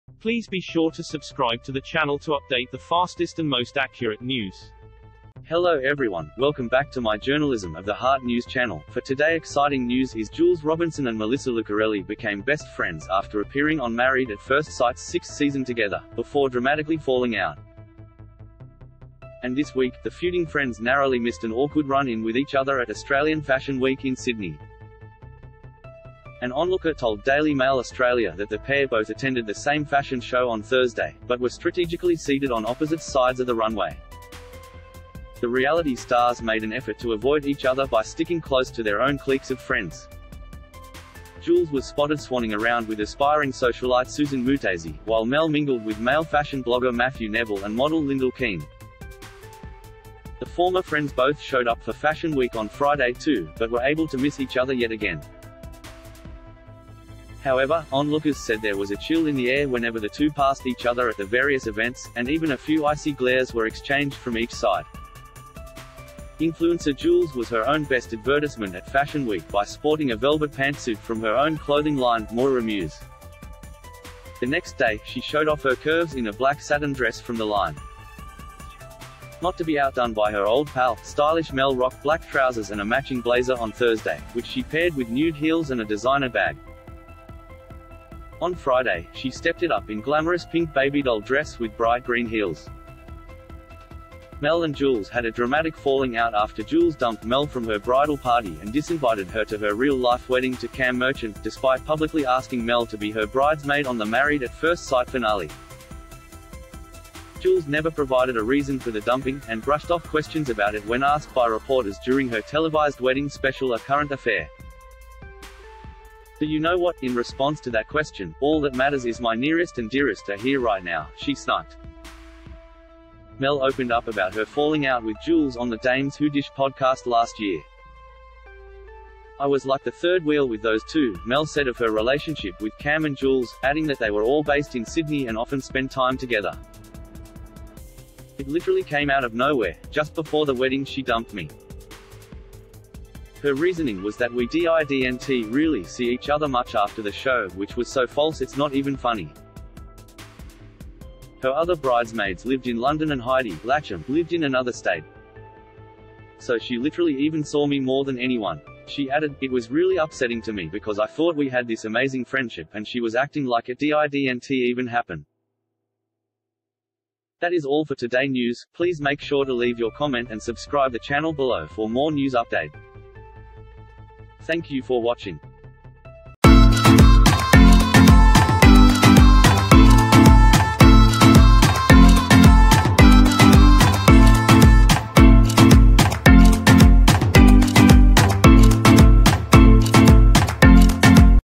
Feuding MAFS brides Jules and Melissa avoid awkward run in at Fashion Week
Feuding Married At First Sight brides Jules Robinson and Melissa Lucarelli narrowly avoid an awkward run-in at Australian Fashion Week following their bitter falling out
Jules Robinson and Melissa Lucarelli became best friends after appearing on Married At First Sight's sixth season together, before dramatically falling out.